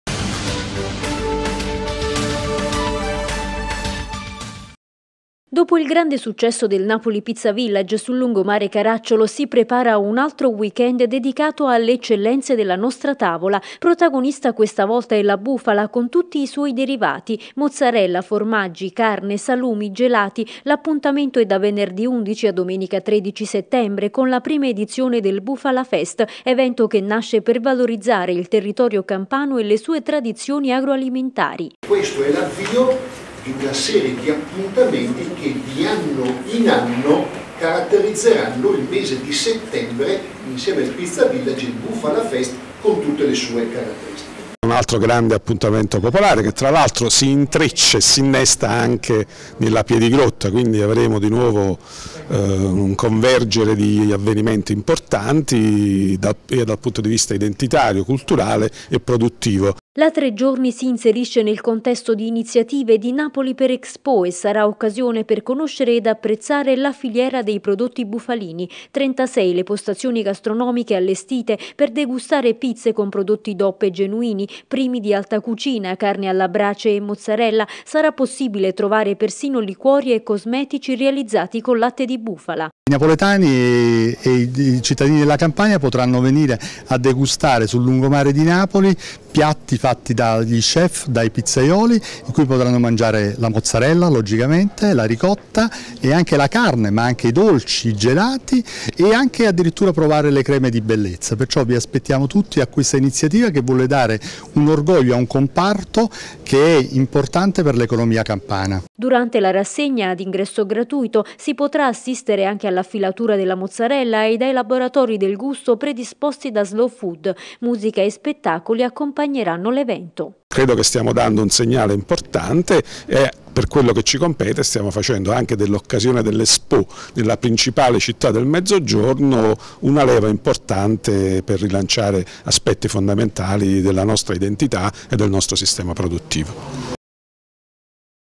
Bufala Fest, da venerdì sul lungomare
Mozzarella, carne, salumi, dolci: sul lungomare Caracciolo arriva la prima edizione del Festival dedicato ai prodotti della filiera bufalina. L'appuntamento è da venerdì 11 a domenica 13 settembre